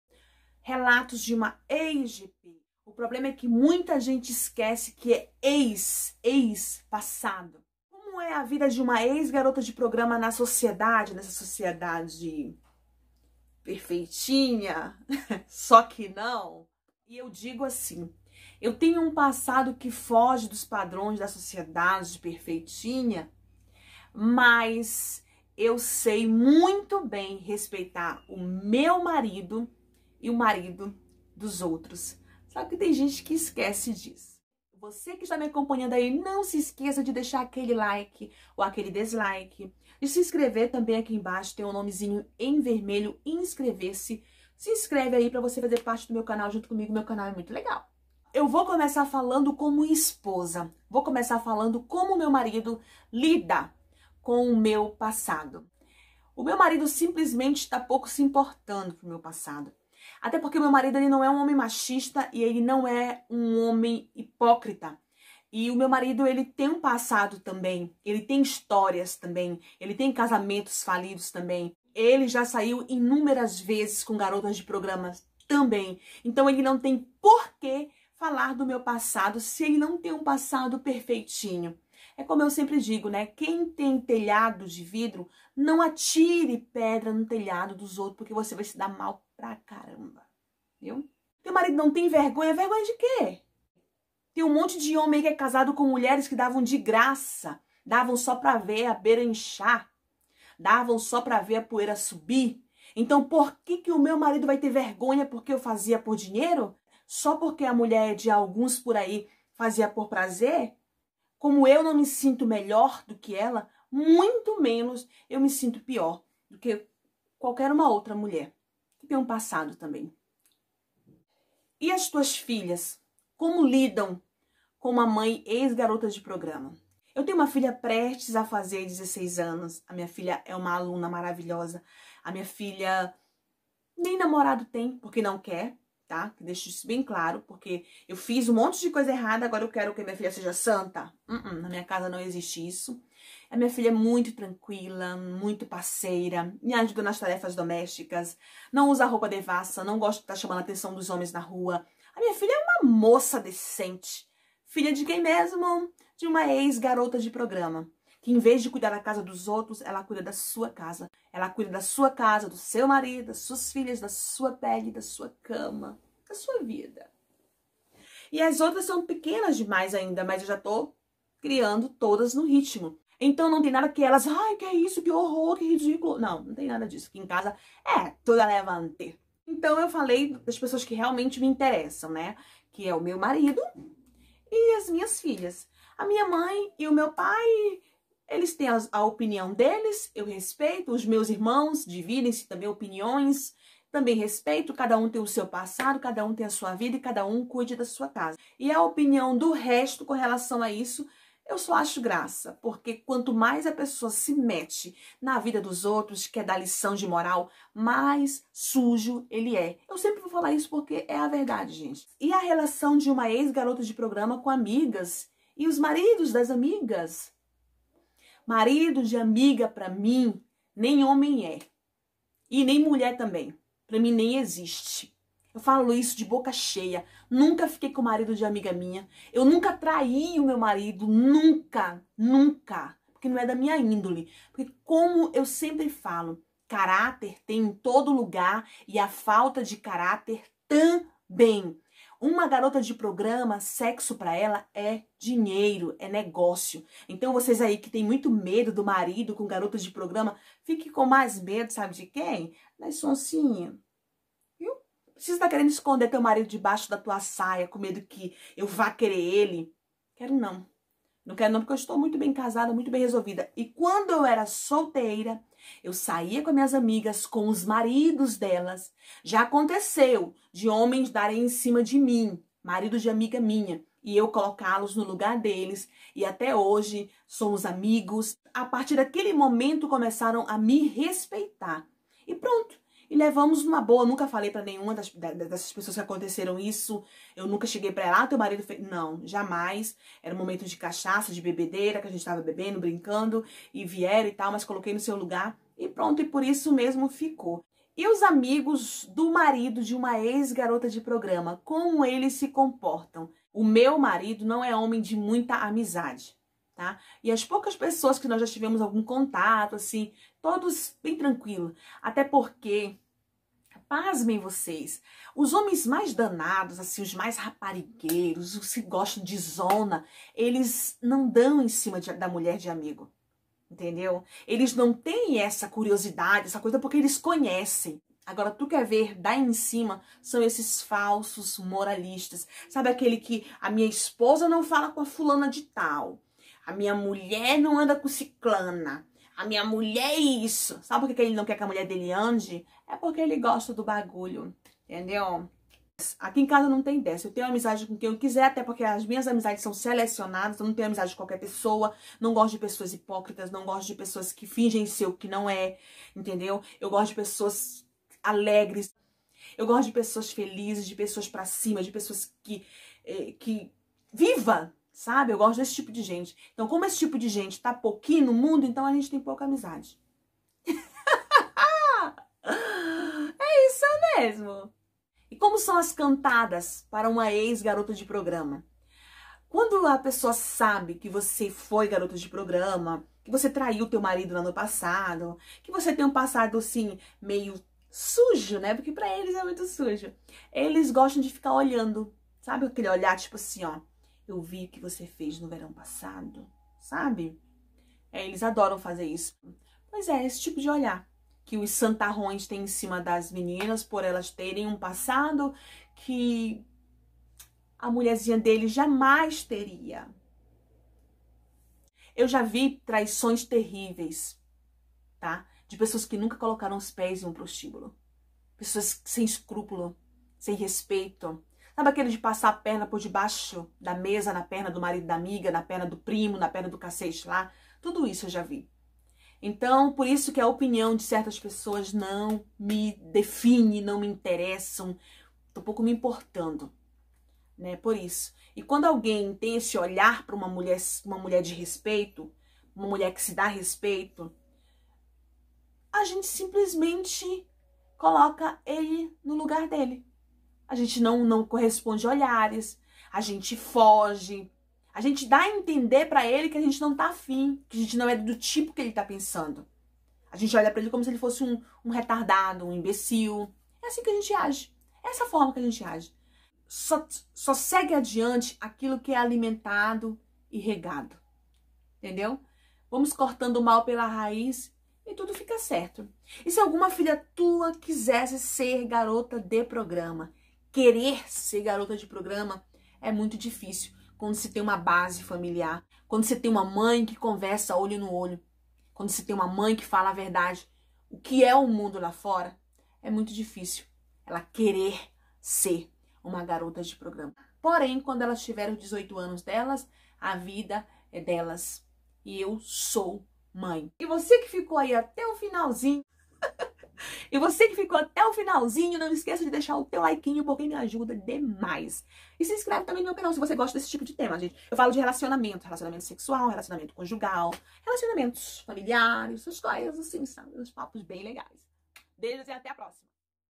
MINHA HISTÓRIA DE PUT@!
No vídeo de hoje falo um pouco das minhas experiências e de como consigo ser esposa e mãe mesmo com um passado tão discriminado.